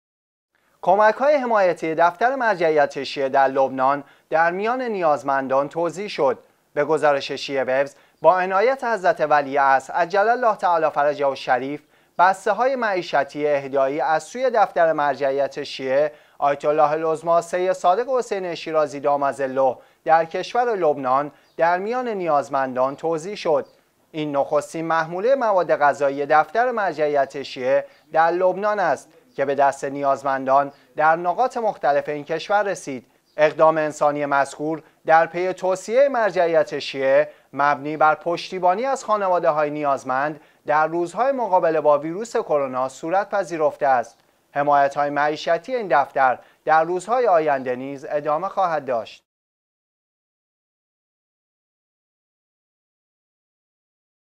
توزیع کمک های مرجعیت شیعه در میان نیازمندان لبنان
کمک های حمایتی دفتر مرجعیت شیعه در لبنان، در میان نیازمندان توزیع شد.
به گزارش شیعه ویوز، با عنایات حضرت ولی عصر عجل الله تعالی فرجه الشریف، بسته های معیشتی اهدایی از سوی دفتر مرجعیت شیعه آیت الله العظمی سید صادق حسینی شیرازی دام ظله در کشور لبنان در میان نیازمندان توزیع شد.
این نخستین محموله مواد غذایی دفتر مرجعیت شیعه در لبنان است که به دست نیازمندان در نقاط مختلف این کشور رسید.
اقدام انسانی مذکور در پی توصیه مرجعیت شیعه مبنی بر پشتیبانی از خانواده های نیازمند در روزهای مقابله با ویروس کرونا صورت پذیرفته است.
حمایت های معیشتی این دفتر در روزهای آینده نیز ادامه خواهد داشت.